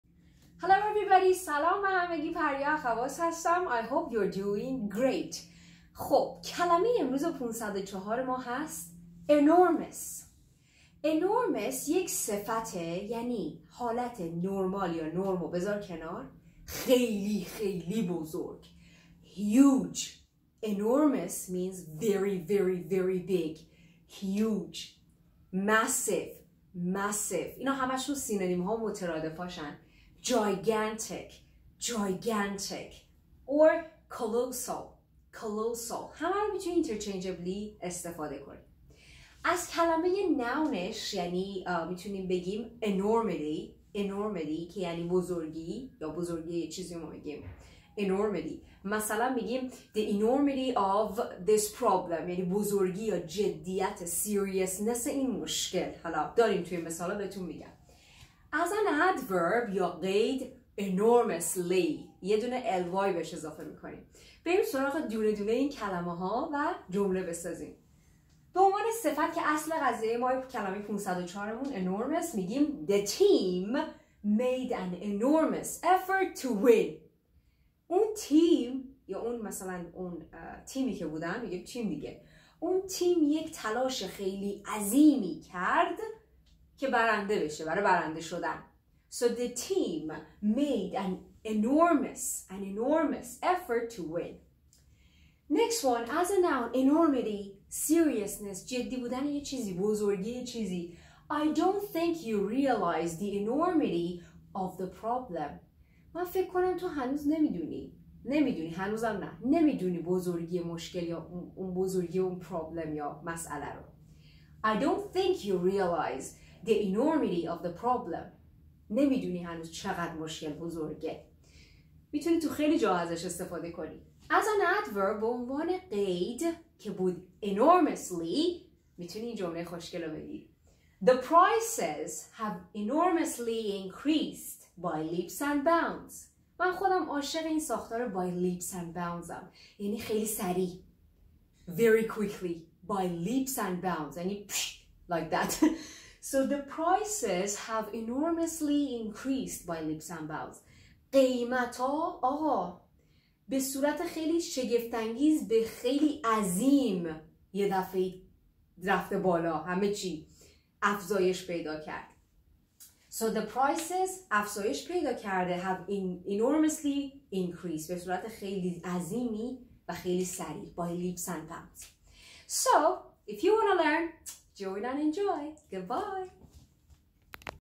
۵۰۴ کلمه ضروری انگلیسی با پریا - Enormous || کلمات و لغات لازم برای انگلیسی پیشرفته
کلمه Enormousاز مجموعه 504 واژه ضروری انگلیسی
504 Absolutely Essential Words
این مجموعه شامل آموزش ۵۰۴ واژه ضروری انگلیسی که از جمله پرکاربرد ترین کلمات در امتحانات آیلتس تافل و GRE می باشند
مدرس پریا اخواص

انگلیسی
انگلیسی حرف زدن
یادگیری انگلیسی
learn English